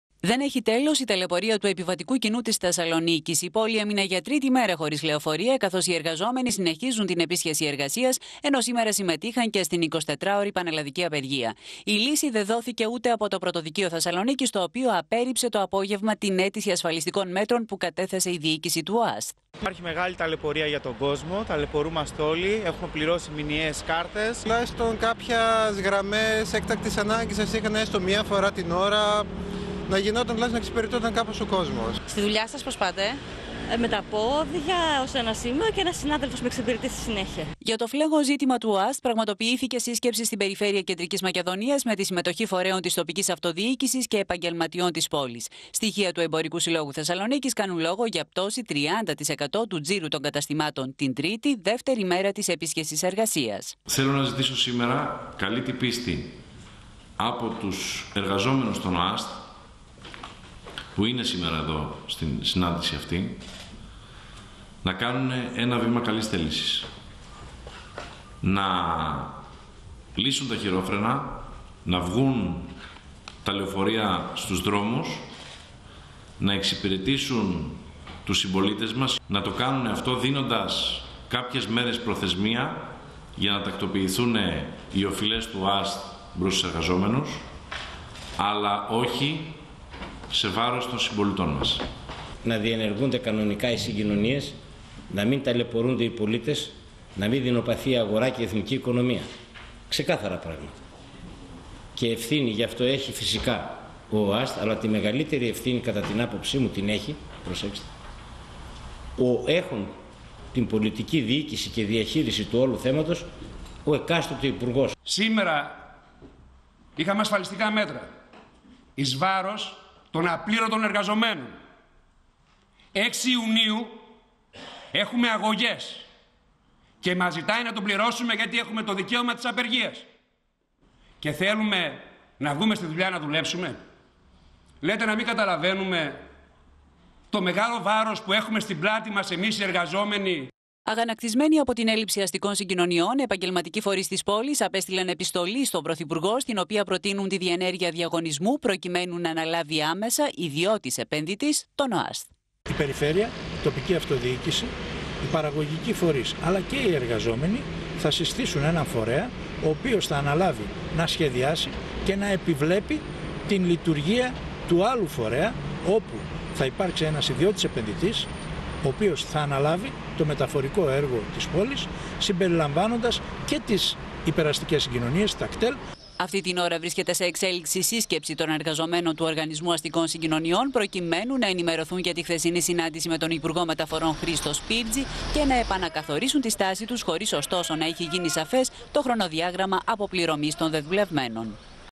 3η ημέρα ταλαιπωρίας χωρίς λεωφορεία για τη Θεσσαλονίκη
Απορρίφθηκε από το Πρωτοδικείο Θεσσαλονίκης το αίτημα της διοίκησης του ΟΑΣΘ να κηρυχθεί άκυρη και καταχρηστική, η επίσχεση εργασίας των εργαζομένων στον Οργανισμό. Έτσι συνεχίζεται η ταλαιπωρία για τους Θεσσαλονικείς καθώς η πόλη παραμένει για τρίτη μέρα χωρίς λεωφορεία. Στο μεταξύ δημόσιο διάλογο για το θέμα του ΟΑΣΘ ζητούν με επιστολή τους προς τον υπουργό Μεταφορών έξι φορείς της επιχειρηµατικής κοινότητας της Θεσσαλονίκης.

ΡΕΠΟΡΤΑΖ ΟΛΓΑ ΝΙΚΟΥ, AΛΕΞΙΑ ΤΖΙΩΝΑ